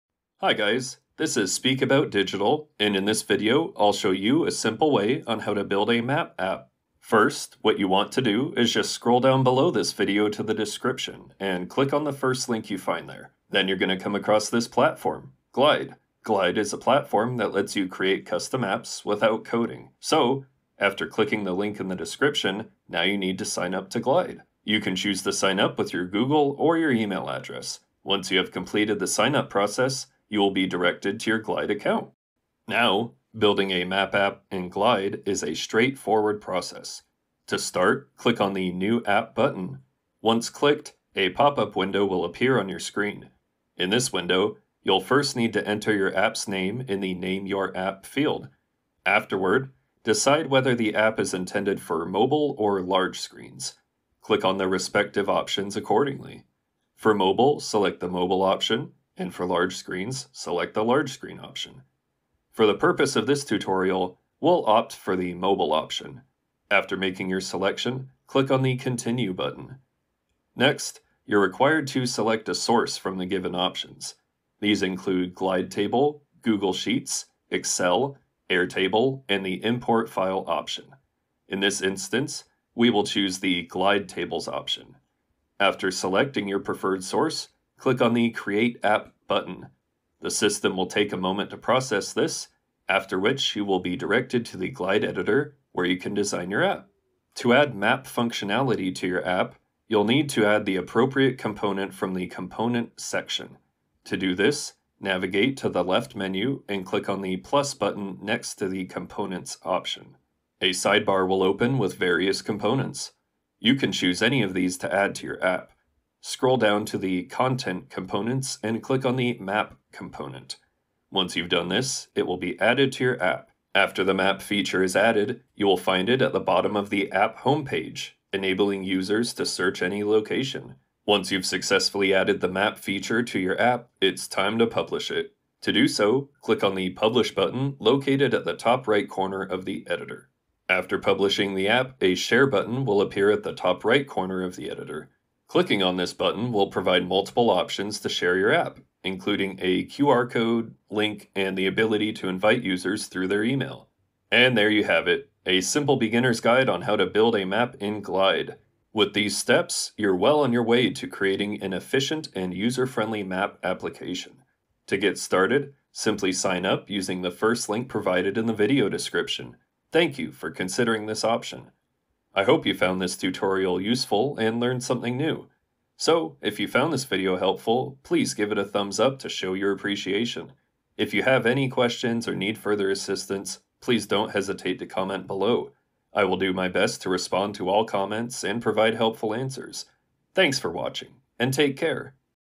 How to build a map app Without Coding
This is my video on How to build a map app Without Coding. I cover the most basic functions.
It's really easy to learn how to do it in just a few minutes by following this super helpful tutorial.